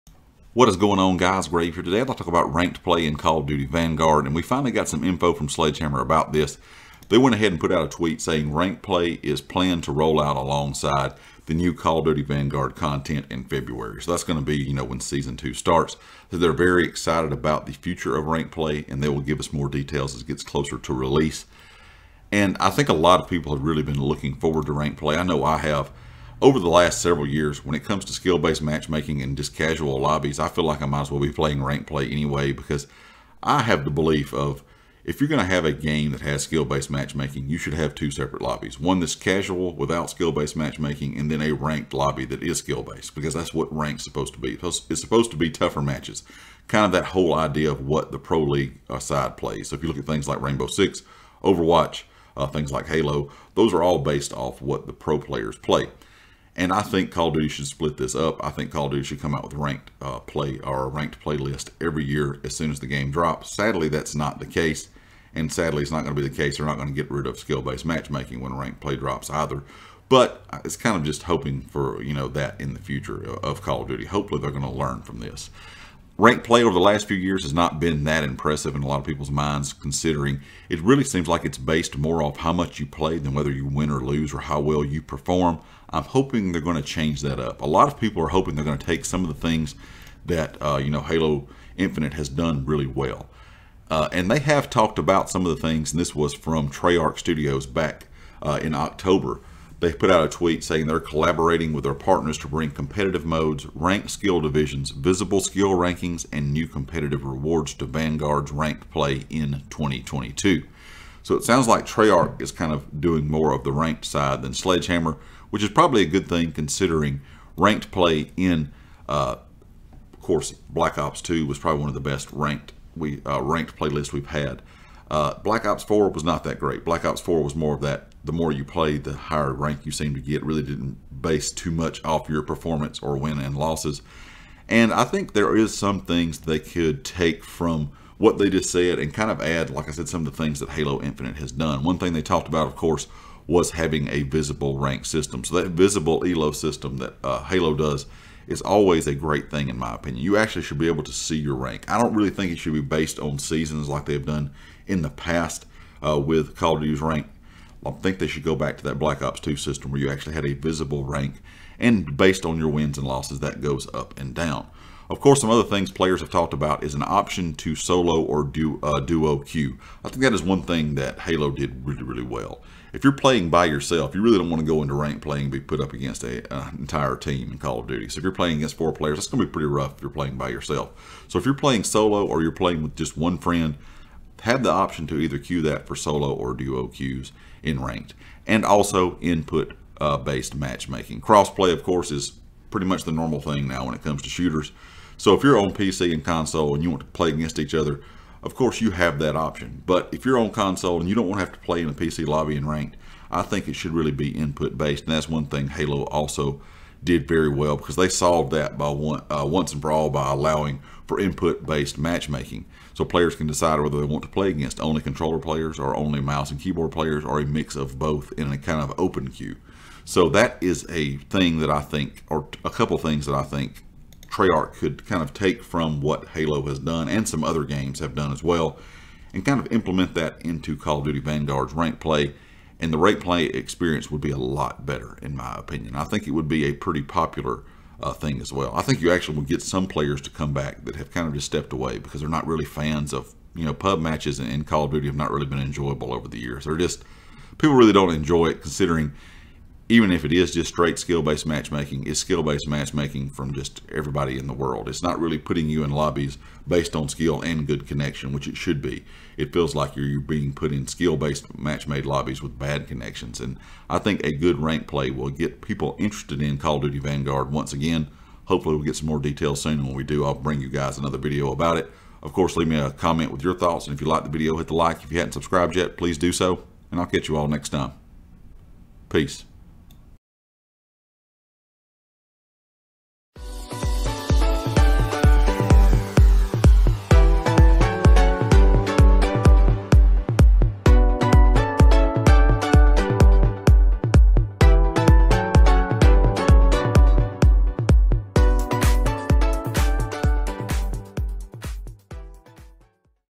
Ranked Play Coming To Call Of Duty Vanguard
Sledgehammer finally gave us some details on when ranked play will be added to call of duty vanguard.